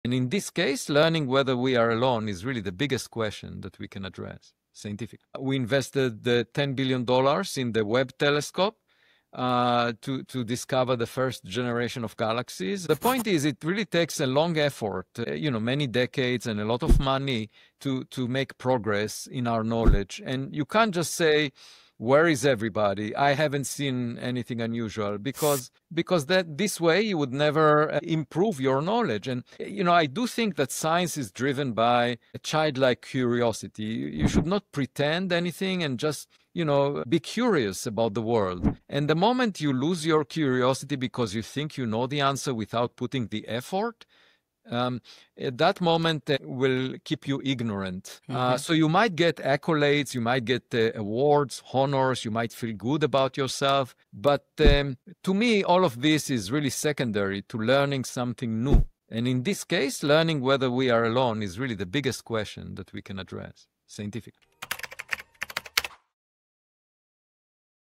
Avi Loeb on 3I/Atlas: Are We Alone?
The buzz around 3A Atlas has everyone asking: “Where is everyone?”
Here, Avi Loeb explains why the biggest question isn’t just if we’re alone—but how we go about finding out.
From $10 billion in Webb Telescope investment to decades-long scientific effort, and why losing curiosity means staying ignorant.